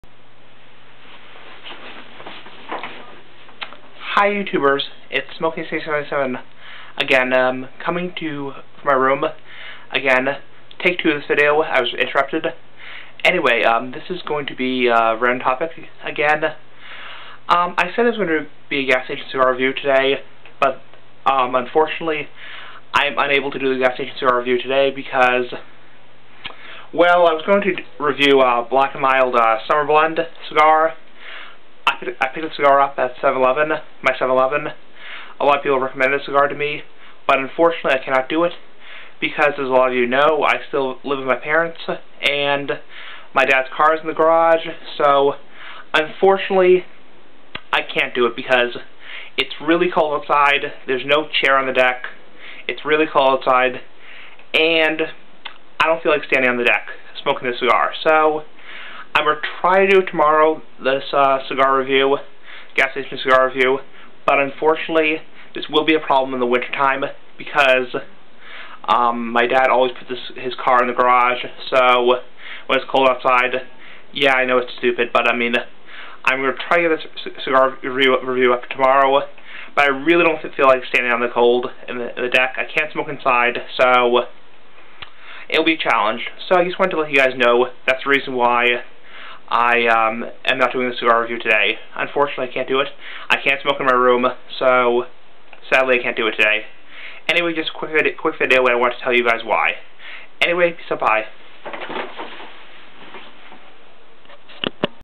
A Problem
Sadly no Gas Station Cigar Review Today this may be a ongoing problem with cigar reviews in the winter time.